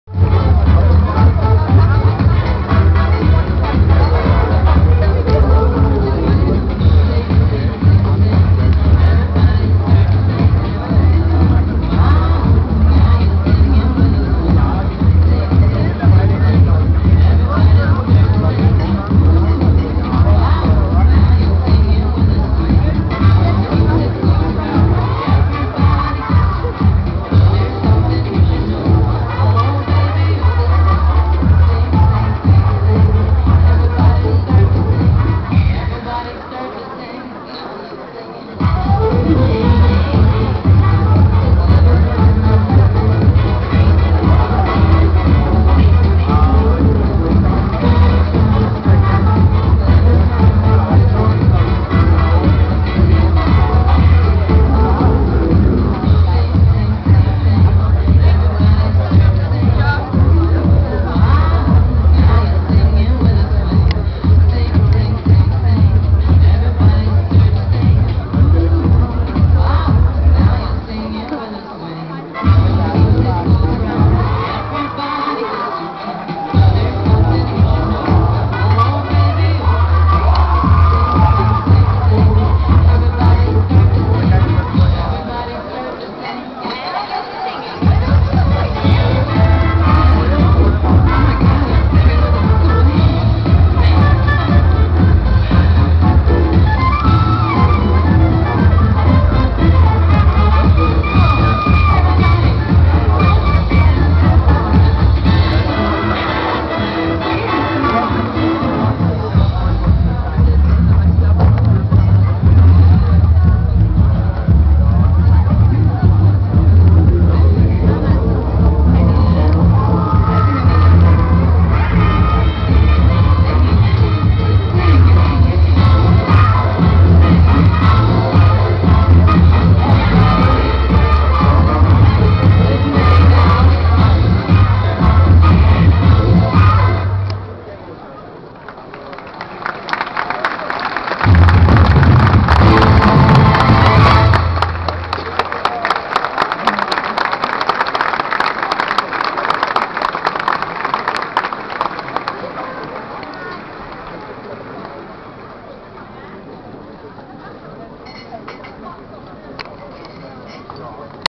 Loooop - Bamberg zaubert 2010 Teil 2
Loooop aus Berlin - Hula Hoop Artistik mit Feuershow